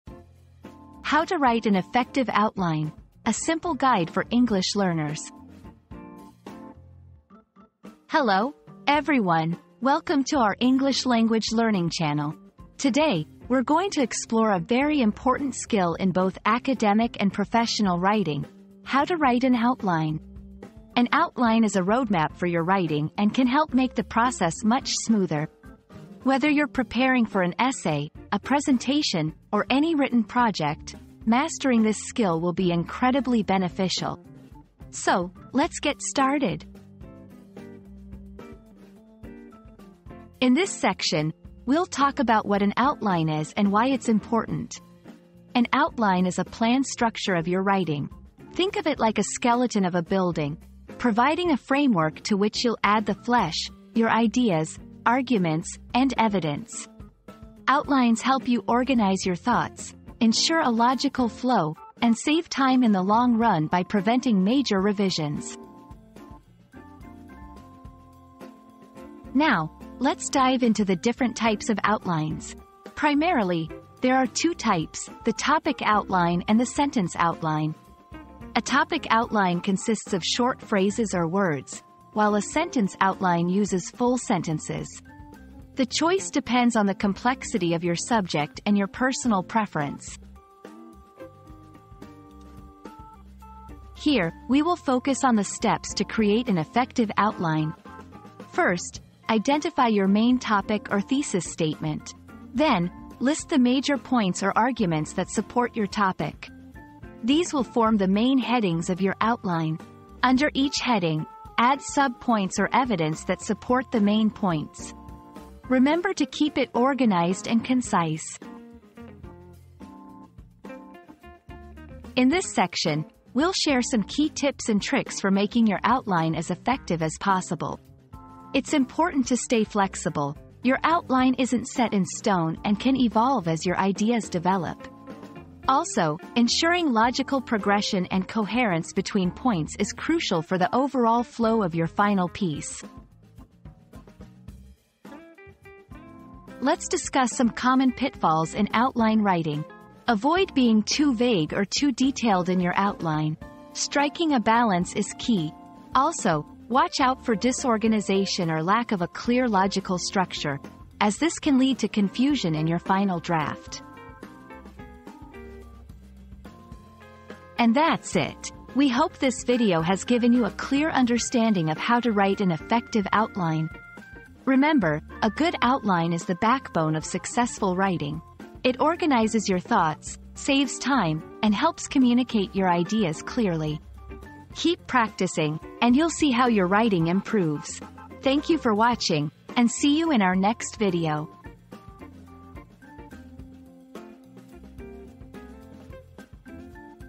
How to Write an Effective Outline: A Simple Guide for English Learners
Mastering Outlines: A Simple Guide for English Learners • Discover the secrets to crafting powerful outlines in English with this simple guide. Learn step-by-step techniques to enhance your writing skills and create effective outlines that will transform your essays and presentations. Take your English proficiency to the next level and become a confident communicator!

00:00 • Introduction - How to Write an Effective Outline: A Simple Guide for English Learners
00:39 • Understanding Outlines
01:08 • Types of Outlines
01:33 • How to Write an Outline
02:01 • Tips for a Strong Outline
02:26 • Common Mistakes to Avoid